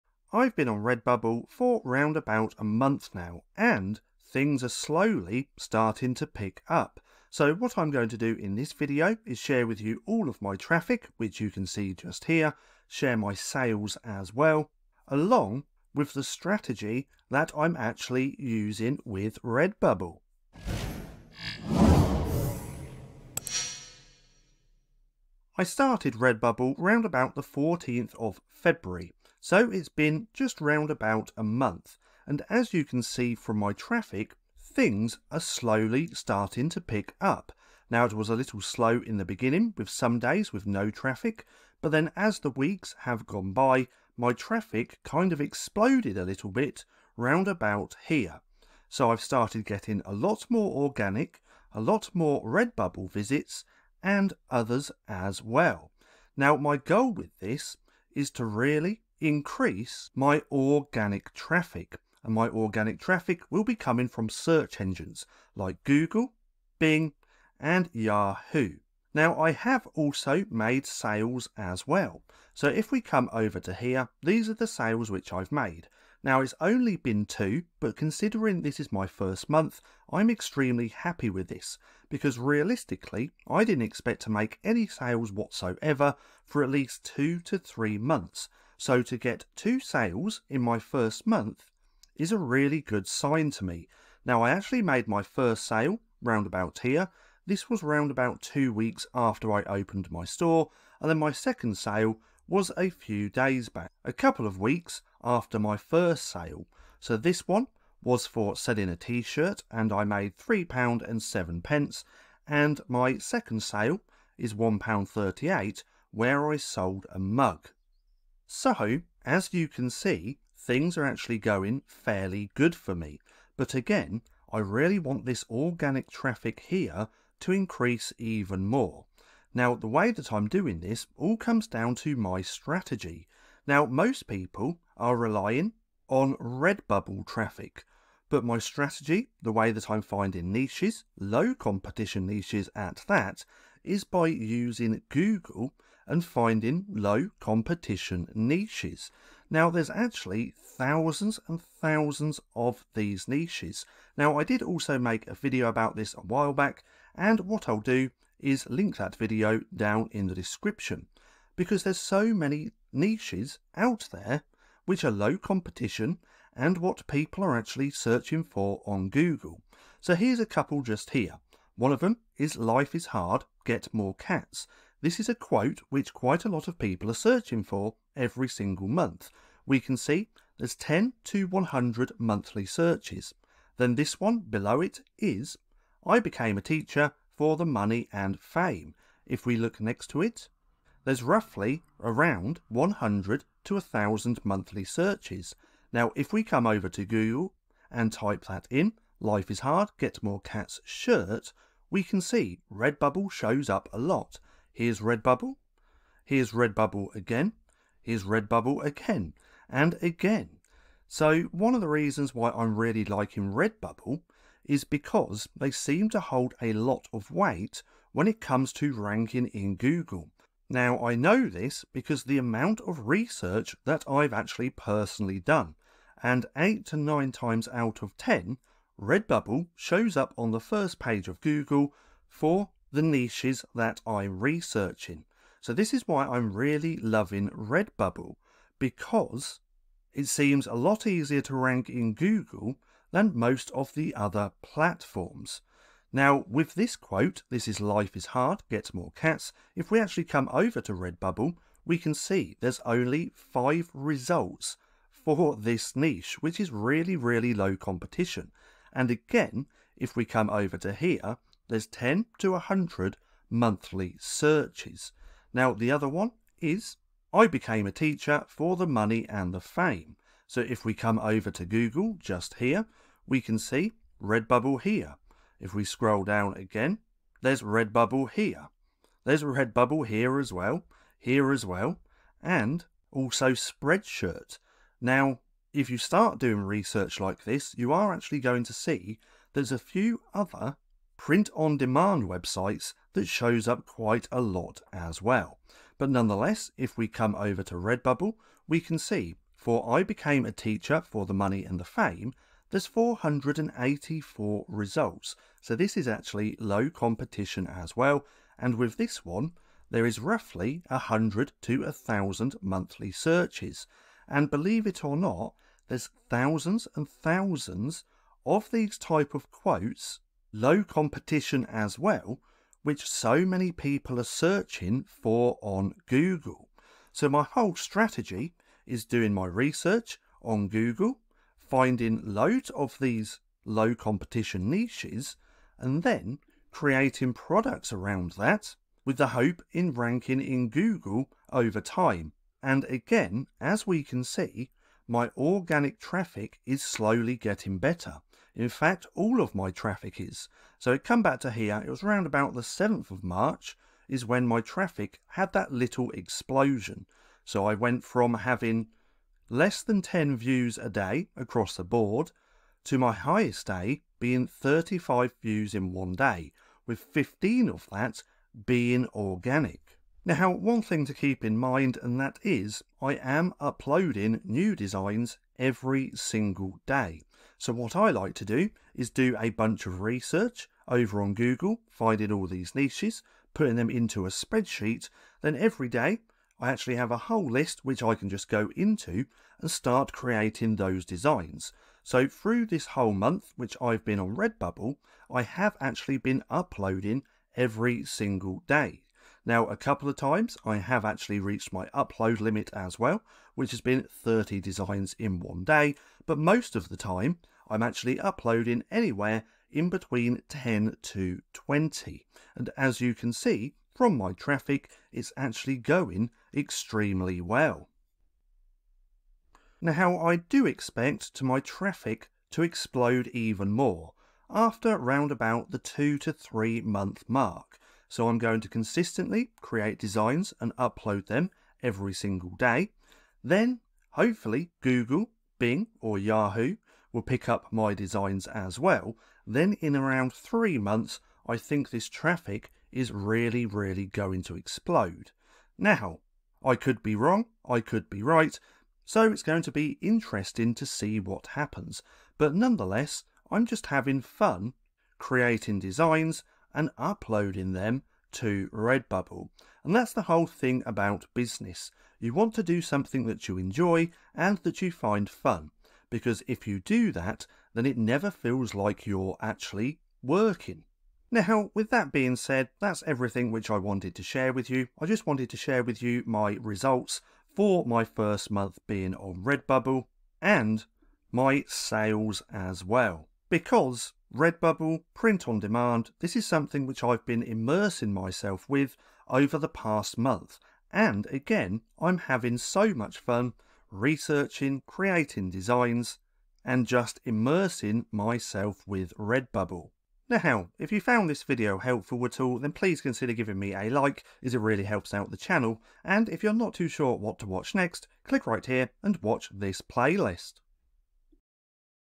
My First Month Selling POD on Redbubble
Talk about my own strategy with Redbubble.